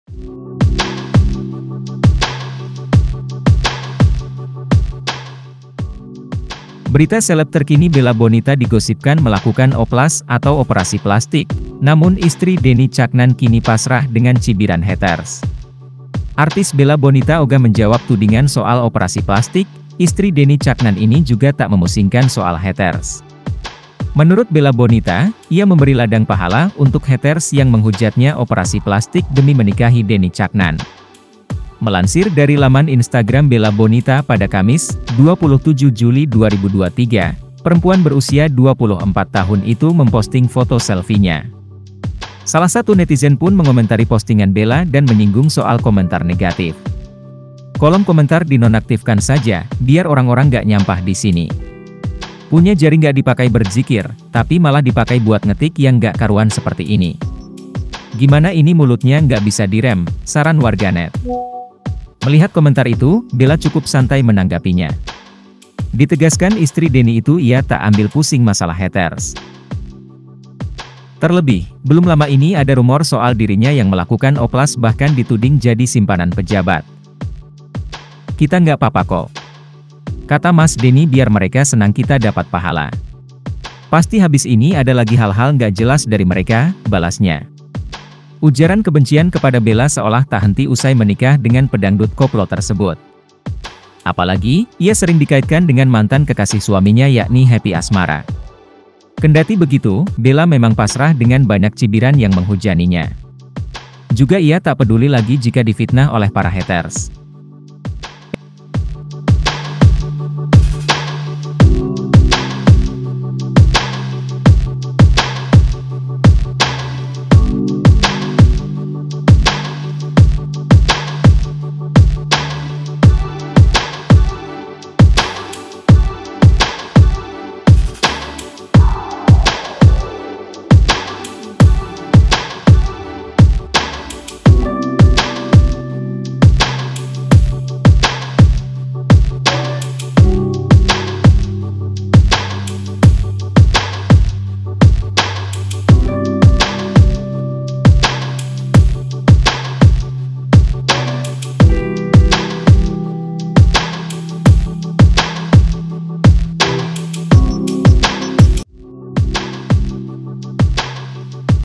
Bella Bonita Pasrah Terima Cibiran Dituding Oplas, Demi Nikahi Denny Caknan
Berita seleb terkini Bella Bonita digosipkan melakukan oplas atau operasi plastik, namun istri Denny Caknan kini pasrah dengan cibiran haters.

Artis Bella Bonita ogah menjawab tudingan soal operasi plastik, istri Denny Caknan ini juga tak memusingkan soal haters.

VP: Darwin